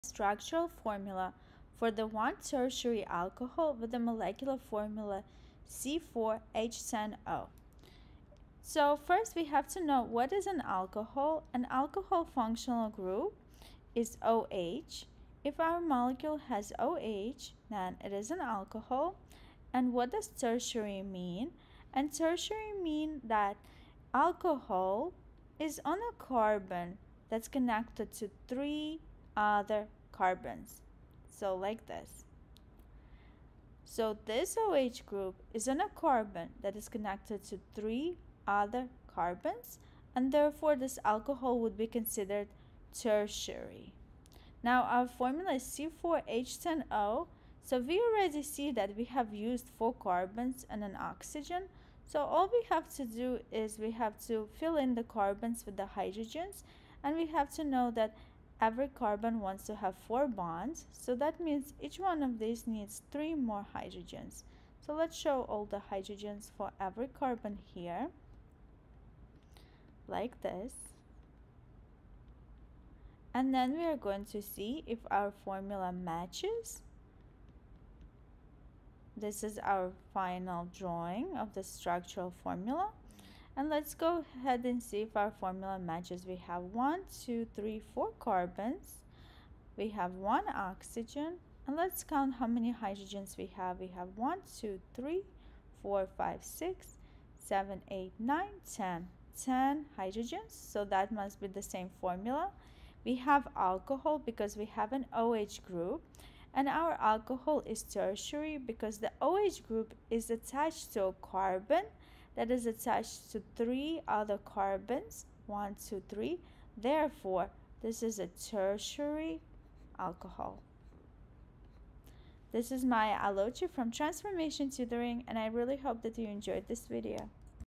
Draw a structural formula for the one tertiary alcohol with the molecular formula C4H10O.
Ready to ace your chemistry class?